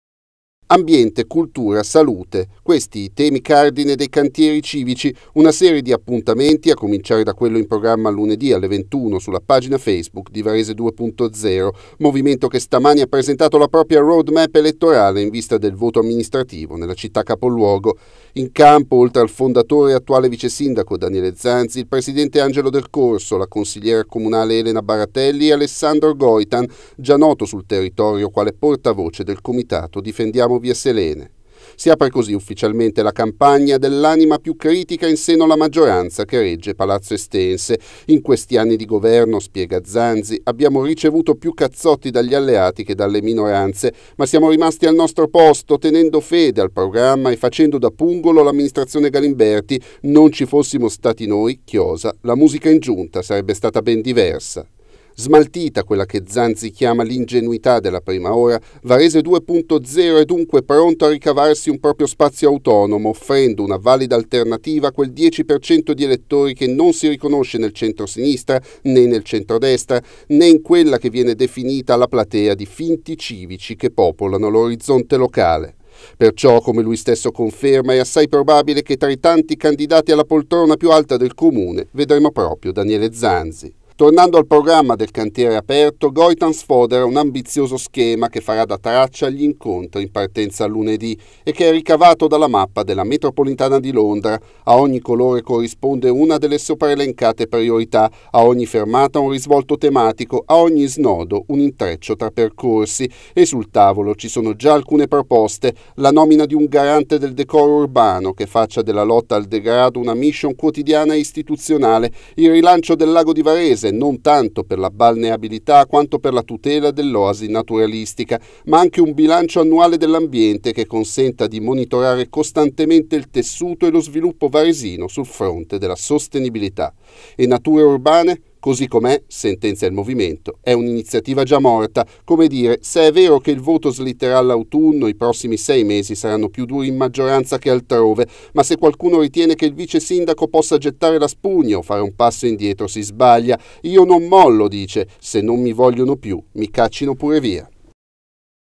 Cantieri elettorali, Zanzi: "Sfido partiti e finti civici"
Daniele Zanzi è pronto a candidarsi sindaco e con la sua Varese 2.0 apre un bimestre di confronto su Ambiente, Cultura e Salute: "La nostra base di consenso è quel 10% di varesini che non si riconosce negli schieramenti" .